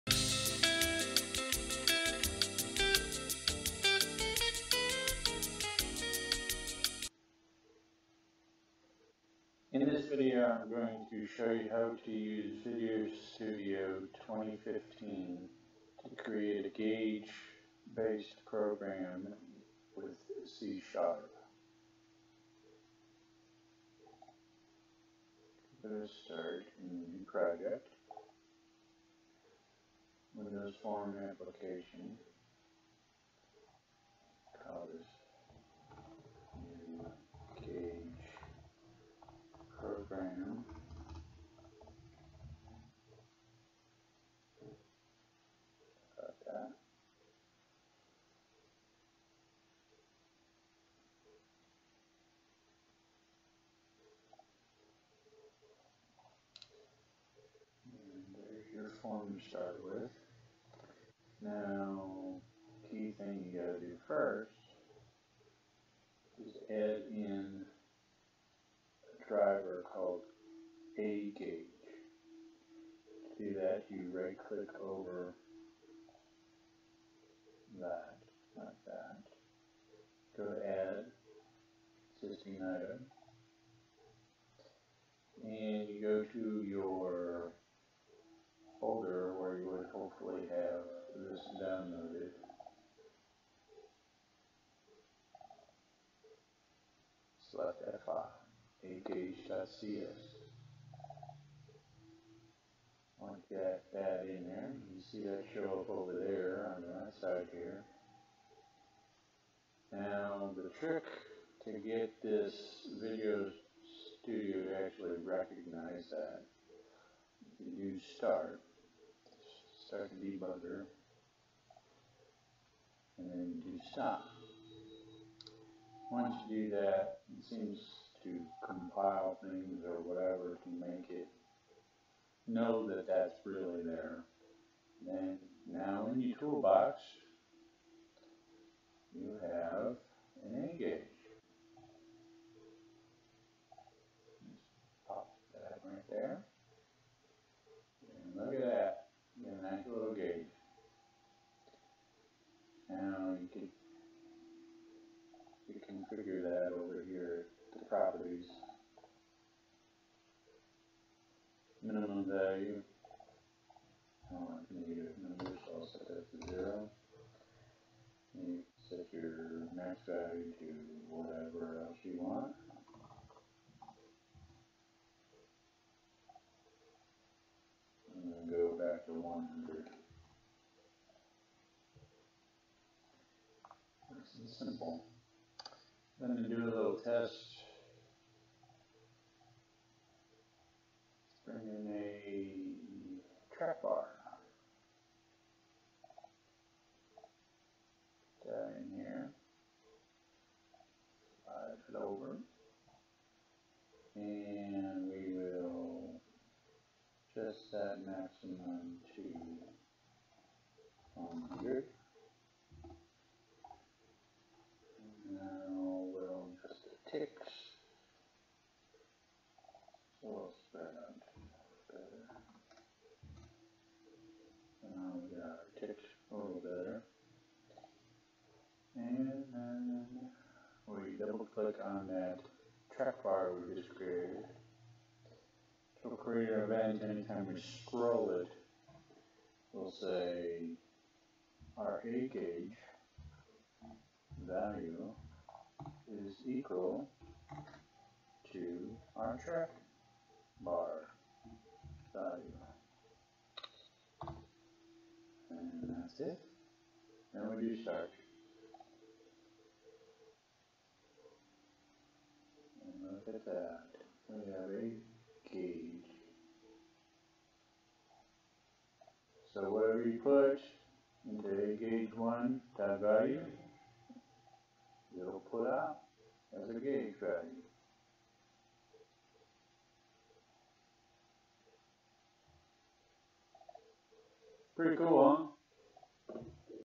How to create a gauge in Visual Studio 2015 C#
This video shows how to use the AGauge source code to easily create a gauge for presenting a value in a Visual Studio 2015 C# program.